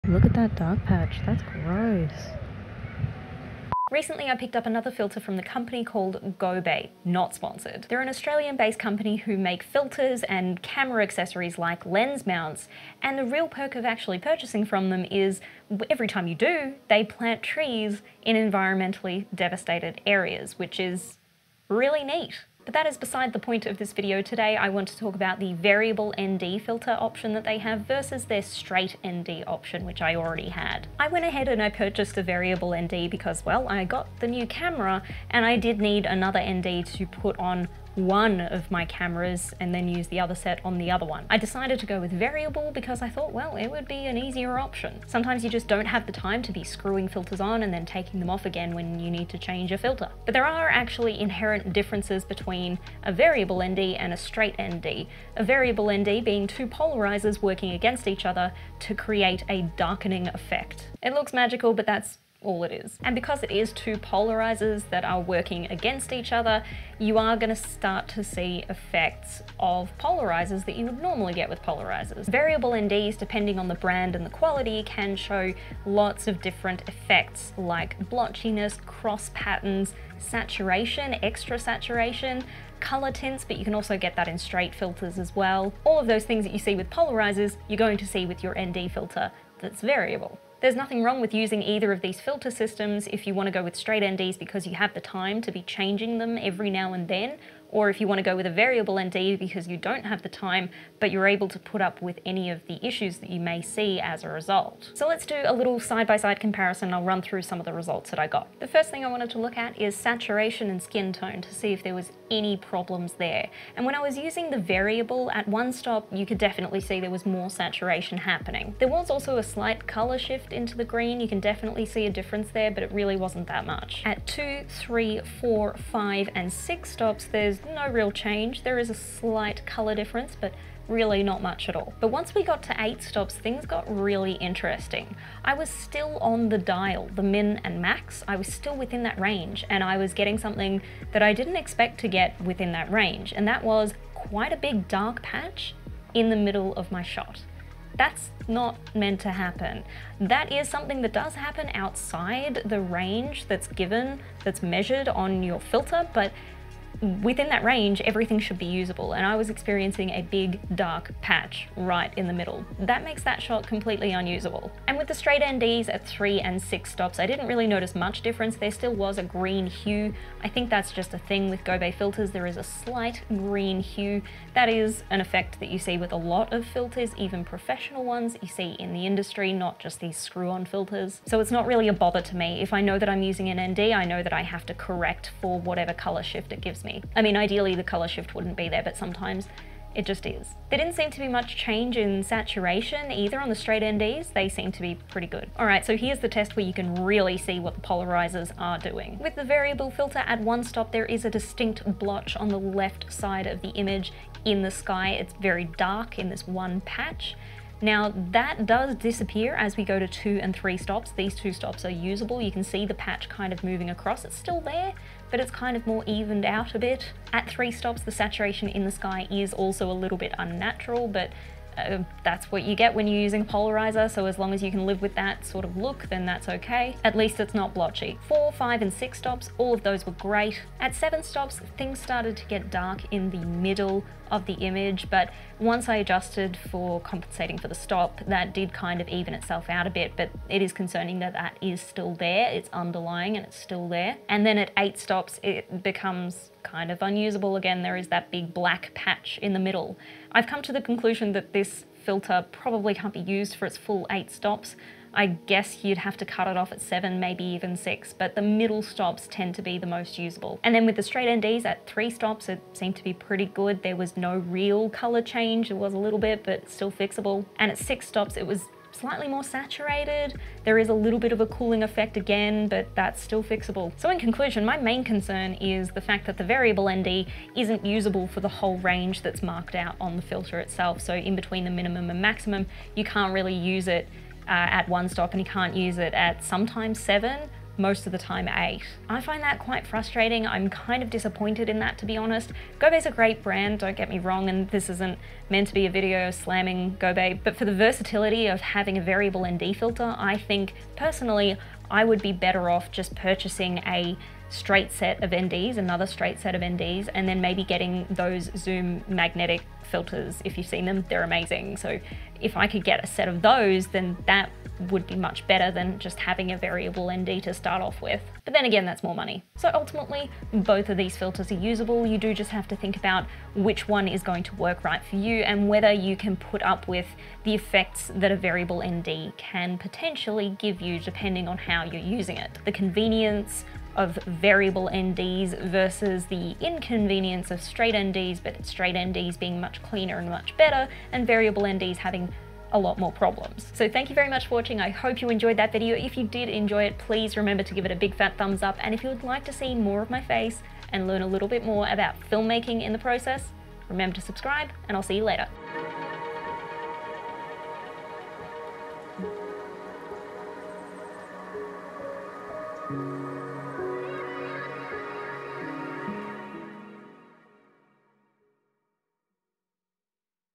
Variable VS Straight ND - Gobe Filter Review
Tiny review and comparrison of the GOBE ND filters. I recently got a Variable ND from them and thought it would be cool to see the differences between Variable and straight and which may be better to use.

Hope you got something from this video and if you have any ideas of things that you want to see in a video, leave a comment below and let me know!

F xx